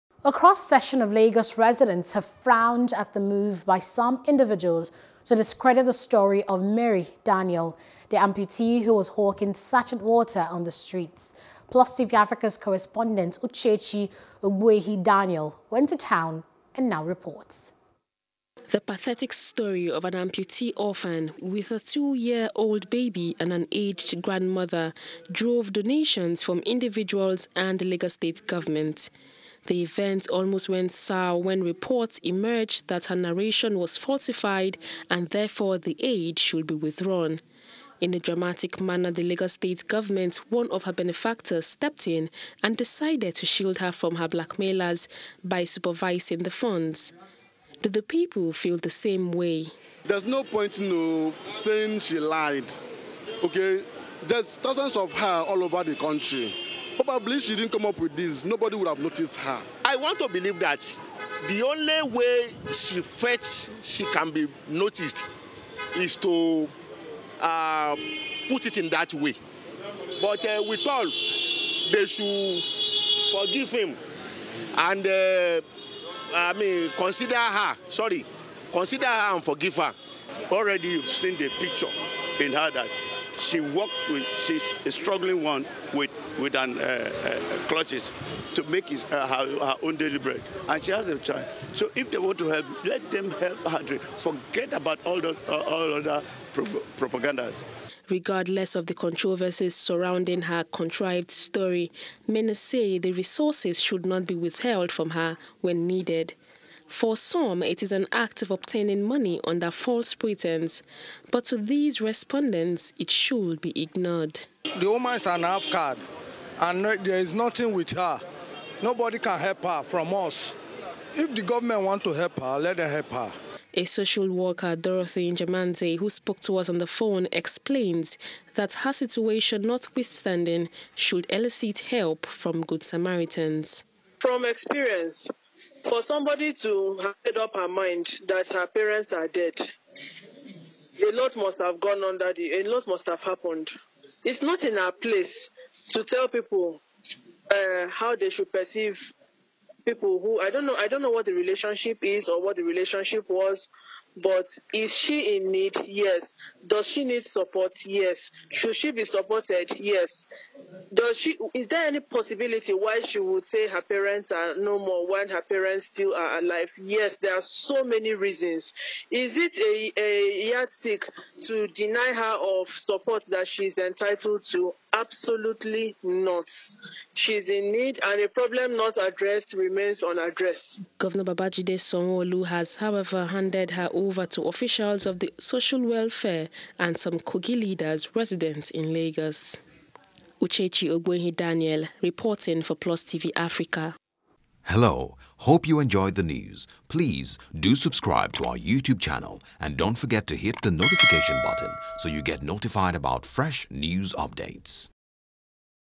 Amputee'ss Ordeal: Lagos Residents Applaud State Govt Gesture Despite Setback | NEWS
A cross-section of Lagos residents have frowned at the move by some individuals to discredit the story of Mary daniel, the amputee who was hawking sachet water on the streets.

Plus tv Africa's correspondent, Uchechi Ogbuehi-Daniel went to town and now reports.